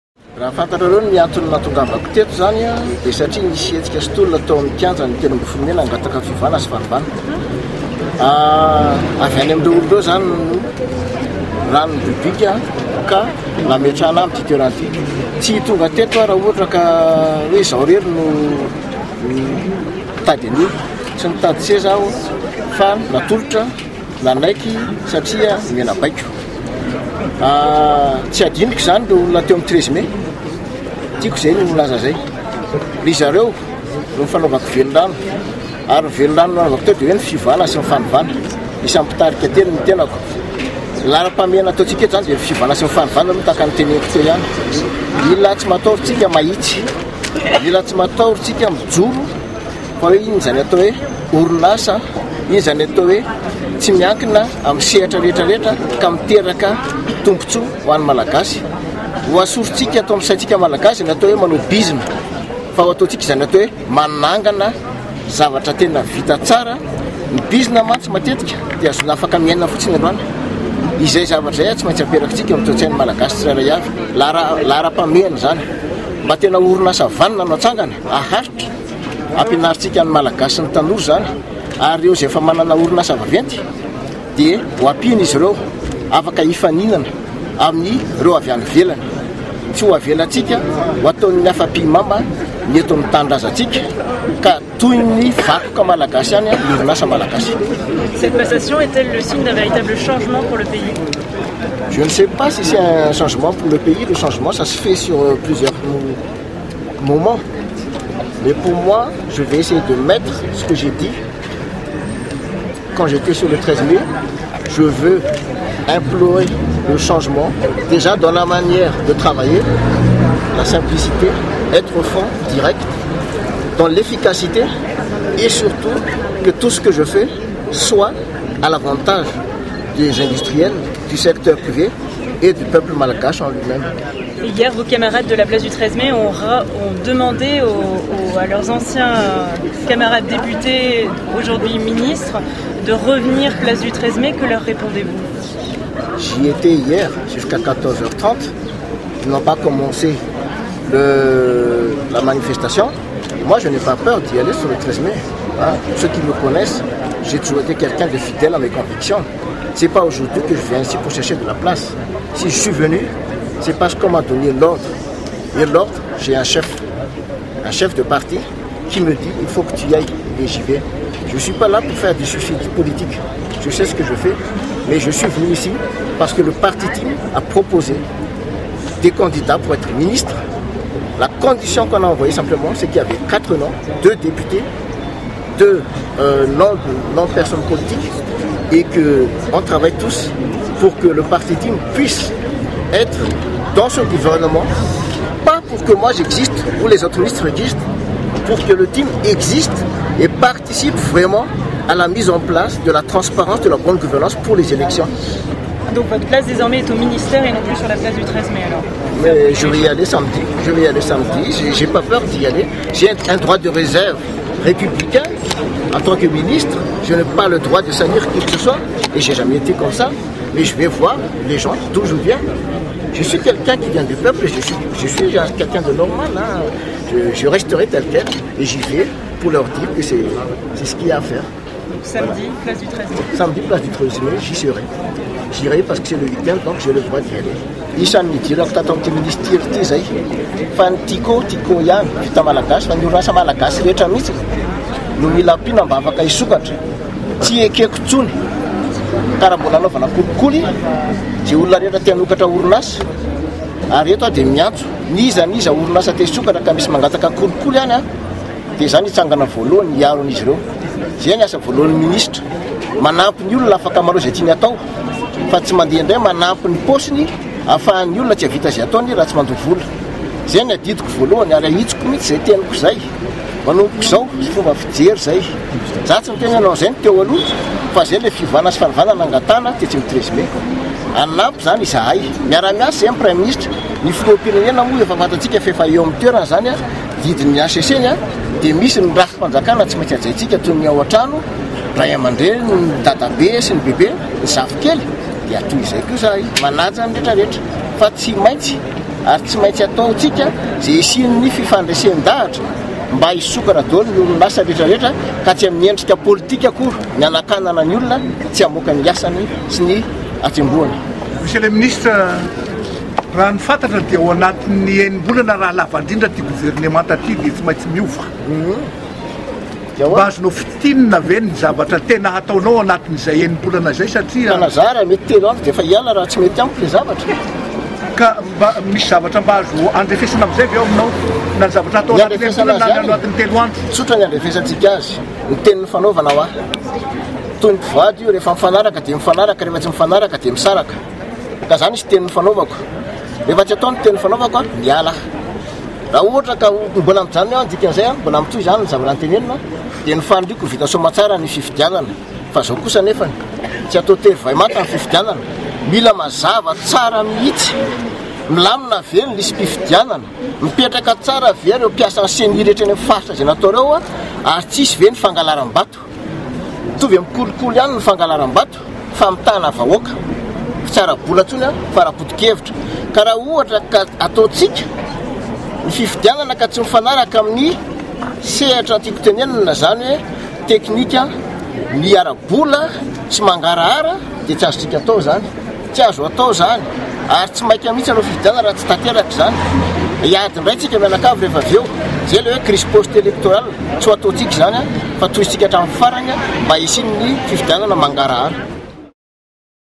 Guy Rivo RANDRIANARISOA ( Minisitry ny Indostria )
De Lire Madagascar : MINISTRA GUY RIVO RANDRIANARISOA: Hanokatra ny orinasa TIKO